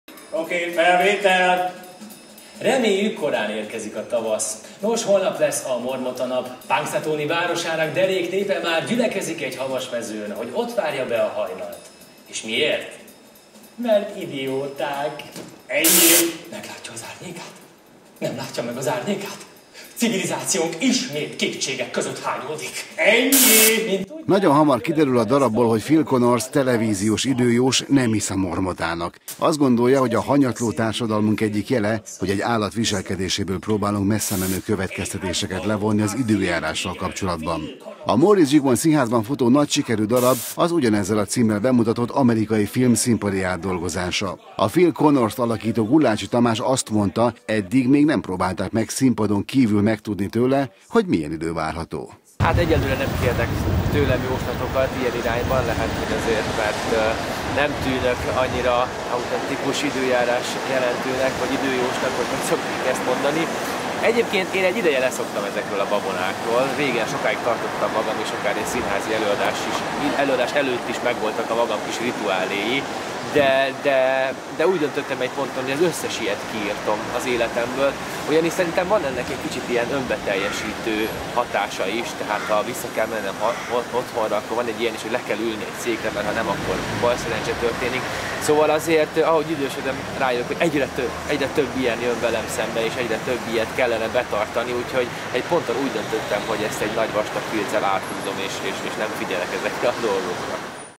Nagy Mormota-nap
Nemcsak Gyertyaszentelő Boldogasszony napja van ma, hanem a Nagy Mormota nap is. Észak-Amerikában ezen a napon húzzák elő odujából a mormotát, aki belesúgja gazdája fülébe, hogy milyen hosszú lesz a tél. Ezt a vicces szokást dolgozta fel az Idétlen időkig című nagysikerű darab, amelyet Magyarországon először a nyíregyházi Móricz Zsigmond Színházban mutattak be. A főszereplő Phil Connors időjóst alakító Gulácsi Tamást kérdeztük arról, hogy ő hisz-e az ilyen jóslásokban.